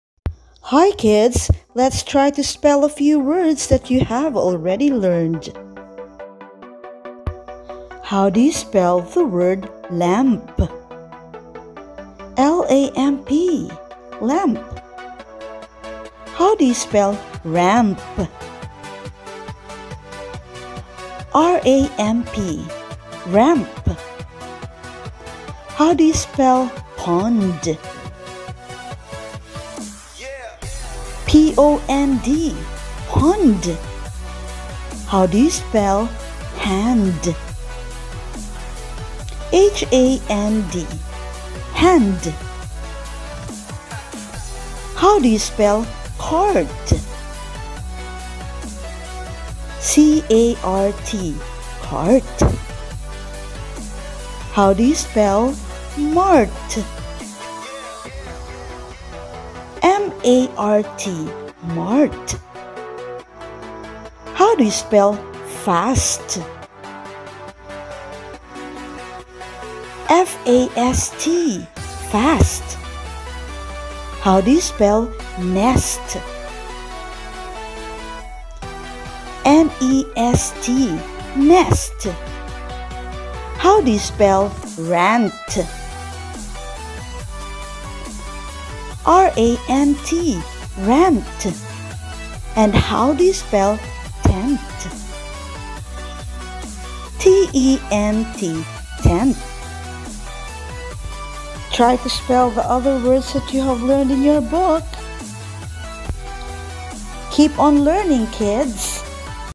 Spelling Words with Ending Blends nd, mp, nt, rt, and st
Kindergarten Lesson in Reading
Dominic Elementary School